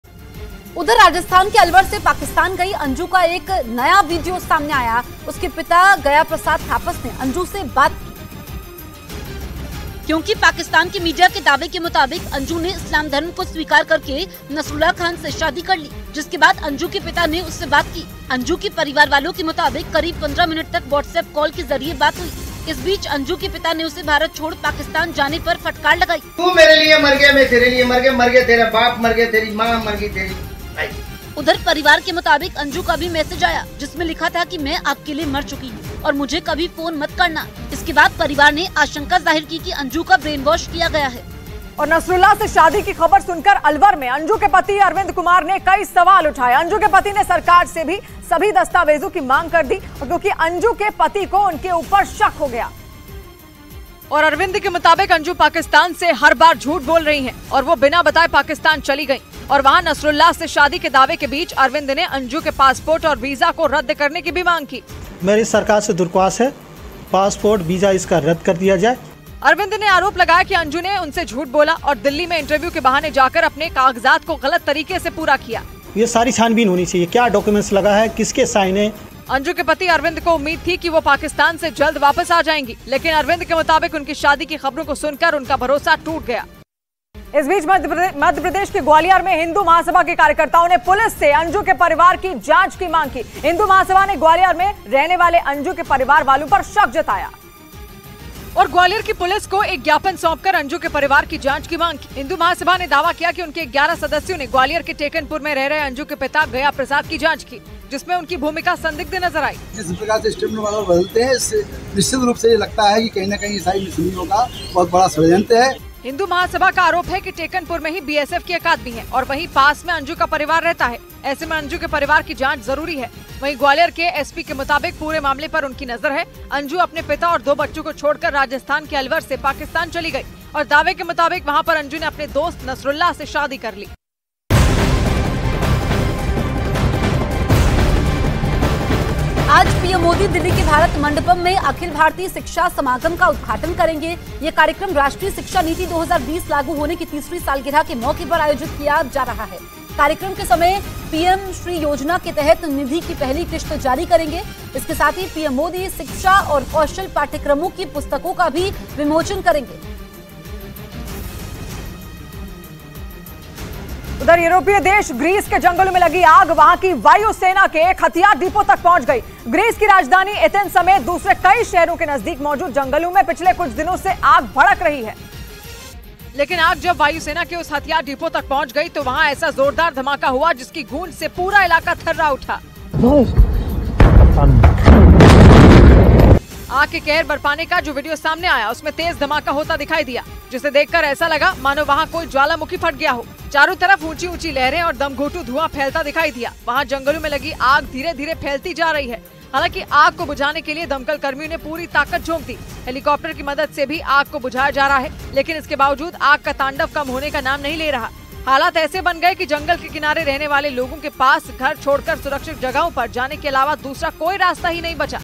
Anju-Nasrullah Love Story : अंजू का एक नया विडियो सामने आया, पिता गया प्रसाद ने की अंजू से बात
Anju-Nasrullah Love Story: A new video of Anju who went to Pakistan from Alwar in Rajasthan has surfaced. Her father Gaya Prasad Thapas talks to Anju

Anju-Nasrullah Love Story : राजस्थान के अलवर से पाकिस्तान गईं अंजू का एक नया विडियो सामने आने। उसके पिता गया प्रसाद थापस ने अंजू से बात की